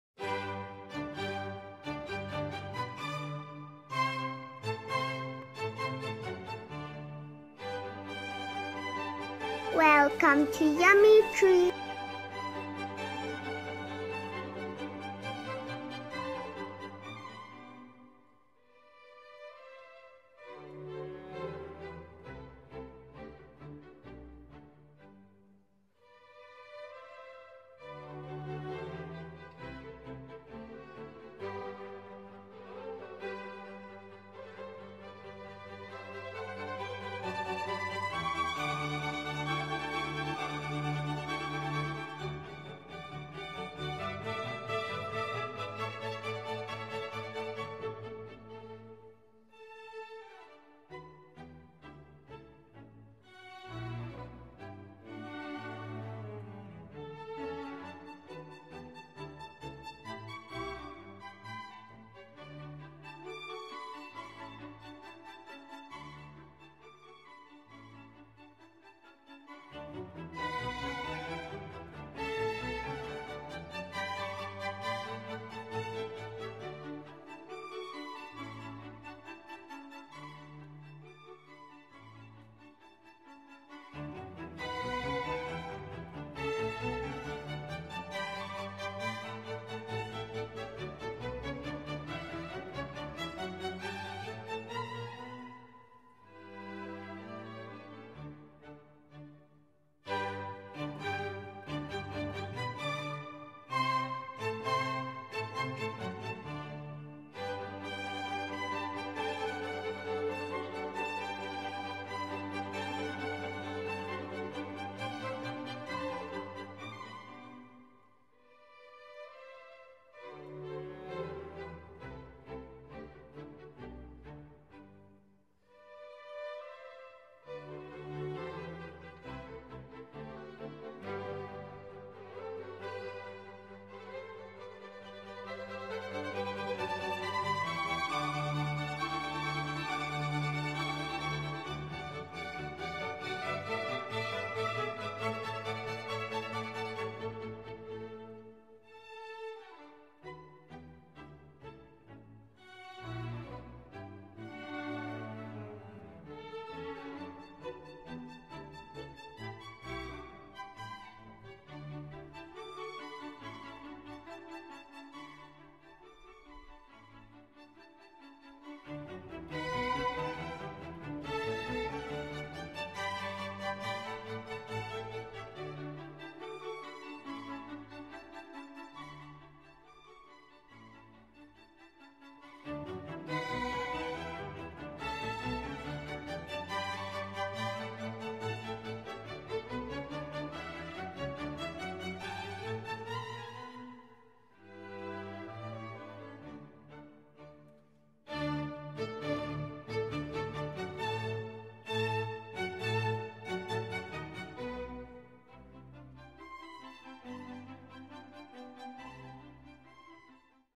Rainbow Sprinkle Sponge Cake Recipe | Birthday Cake Recipe
Rainbow Sprinkle Sponge Cake or somely Birthday Cake is fun to make and is delicious and rich in rainbow sprinkles which gives the cake a colorful streak . Vanilla sponge cake filled to the brim with rainbow sprinkles and finished off with your favorite frosting will give you a beautiful rainbow sprinkles cake without which no celebration is complete .Learn how to make it completely from scratch using this detailed homemade recipe.